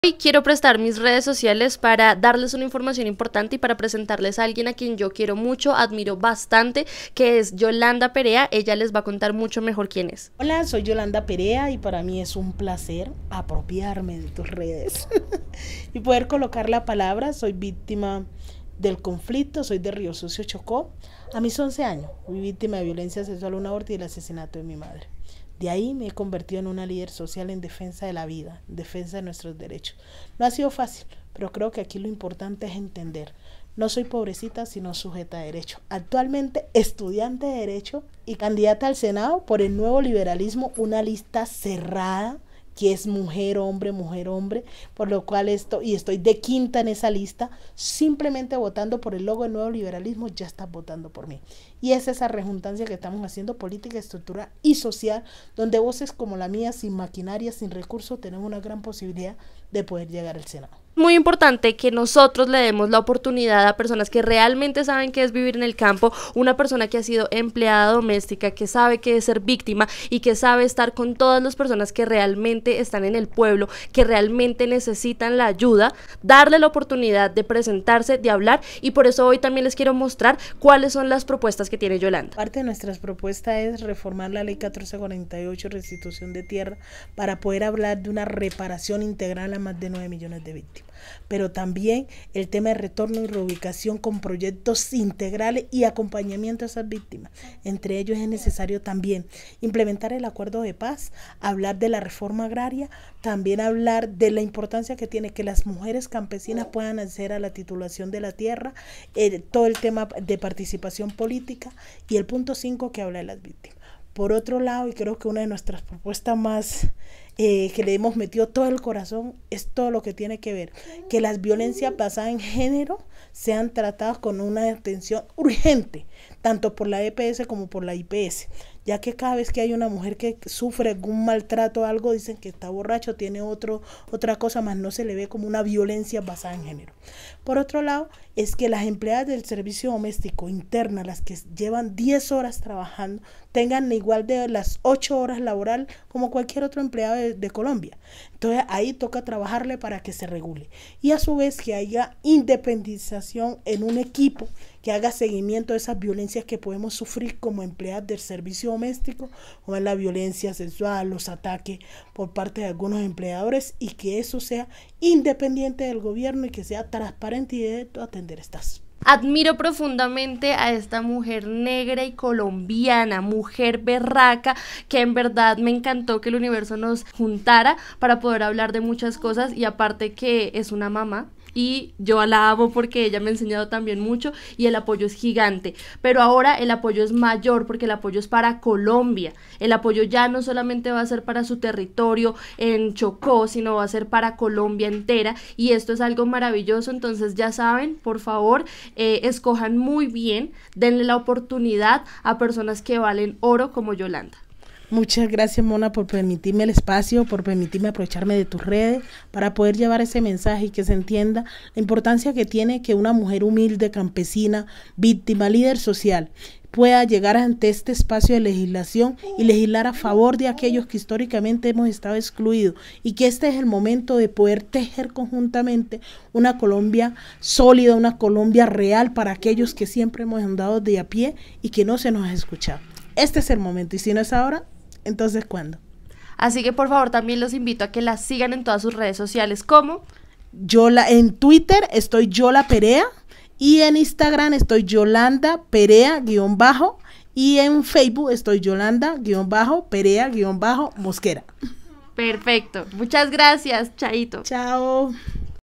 HABLANDO CON YOLANDA PEREA LIDERESA DEL CHOCÓ COLOMBIA | La Mona Soy Yo
Si no sabes por quien votar aqui te dejo una candidata YOLANDA PEREA
Este 13 de marzo los invito a votar a consciencia, dándole la oportunidad a personas y mujeres de a pie, que saben realmente lo que es vivir en lugares olvidados de Colombia 🇨🇴
Un verdadero cambio se logra votando por los candidatos que son del pueblo para el pueblo.
Les presento a @yolaperea_ mujer berraca, negra y Colombiana, que hoy pide tu voto para el senado con el Nuevo Liberalismo.
Ella es una lideresa social de Riosucio Chocó, víctima del conflicto y de la violencia sexual, que durante años ha luchado por las mujeres víctimas de violencia, ha sobrevivido con menos del mínimo para alimentar a su familia. Y hoy ha llegado a ser candidata para el senado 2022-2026 porque cree que podemos lograr un real cambio en Colombia 🇨🇴
Gracias Yolanda